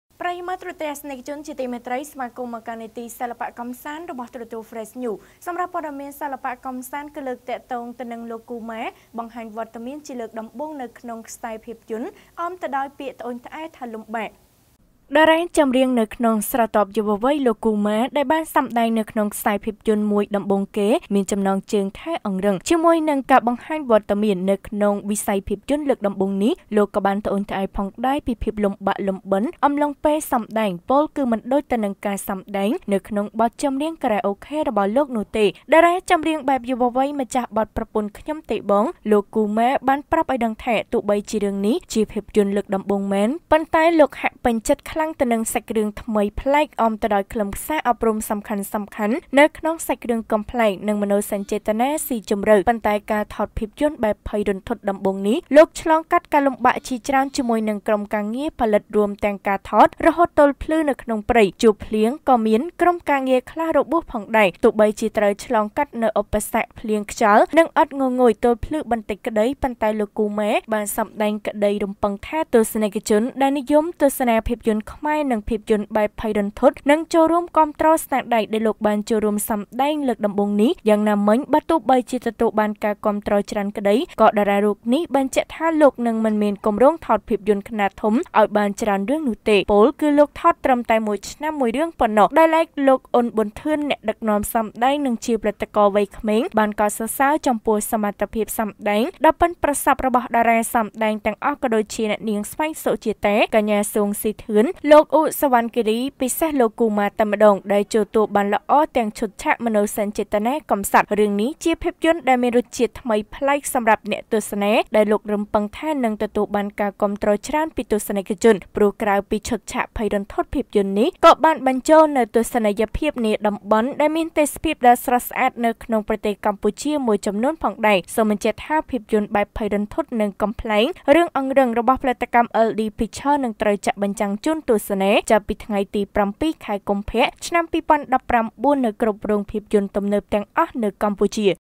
លោក ​ គូ​ម៉ា បង្ហាញវត្តមាន ​ក្នុង​ខ្សែភាពយន្ត​​លើកដំបូង អមដោយពាក្យត្អូញត្អែរថា «លំបាក»
លោក ​ គូ​ម៉ា បង្ហាញវត្តមាន ​ក្នុង​ខ្សែភាពយន្ត​​លើកដំបូង អមដោយពាក្យត្អូញត្អែរថា